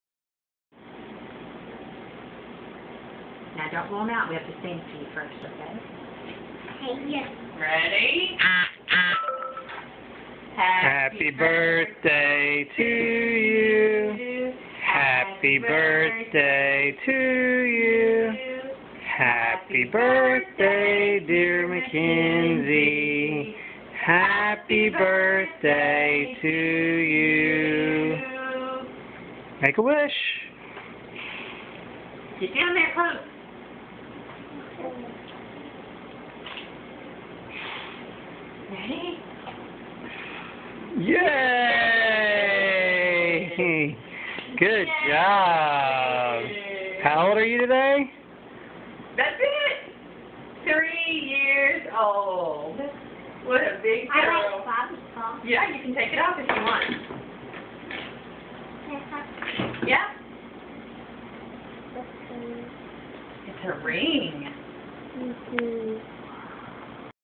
Making A Wish
Mackenzie teaching Braxton how to make a wish and blow out the candles.